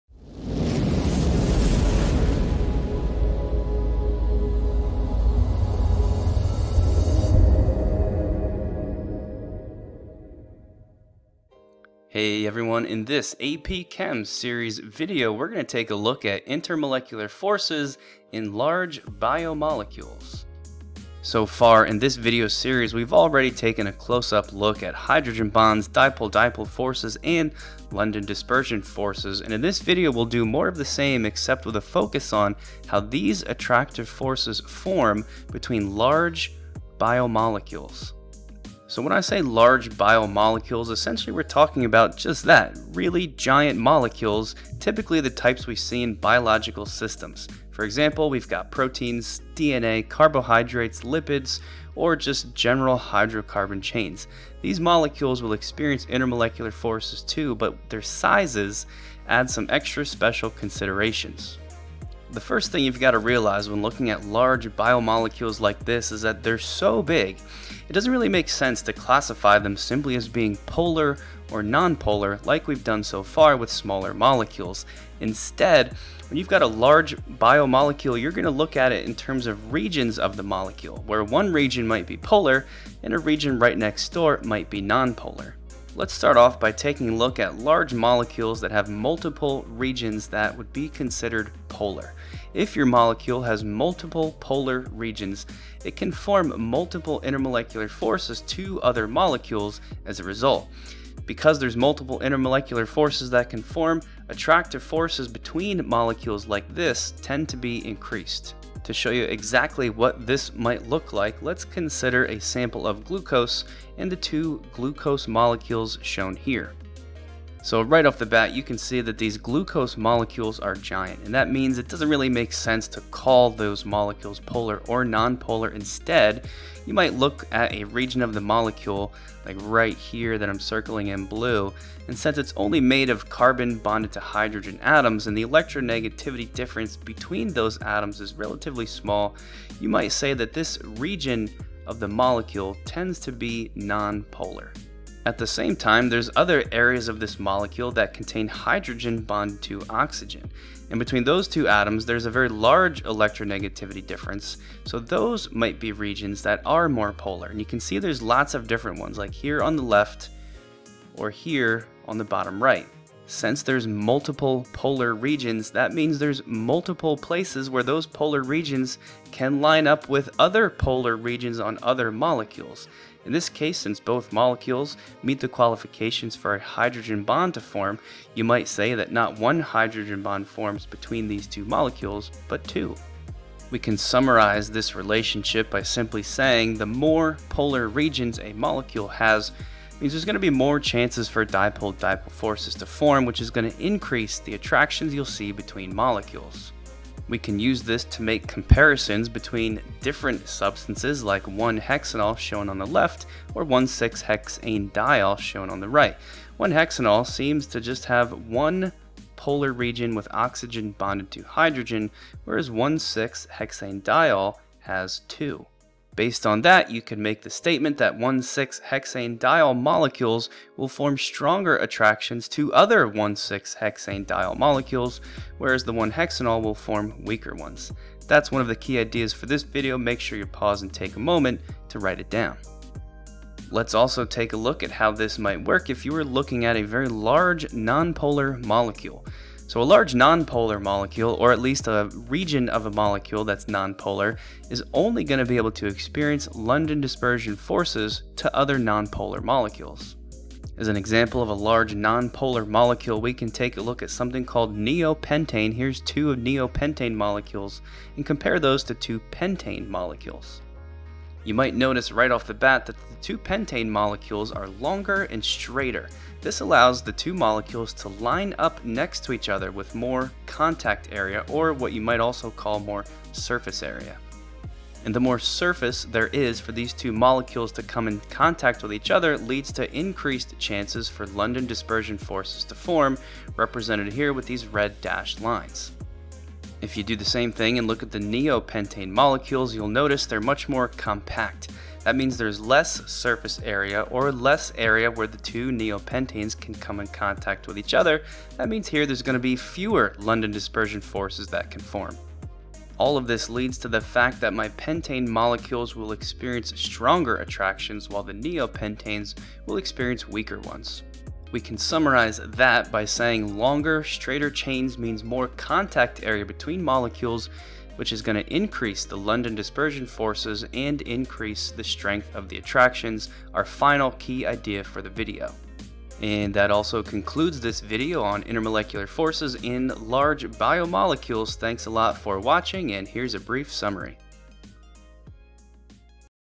Intermolecular Forces between Large Molecules
In this video, I'll tell you about the dipole-dipole and London dispersion forces that form between large molecules.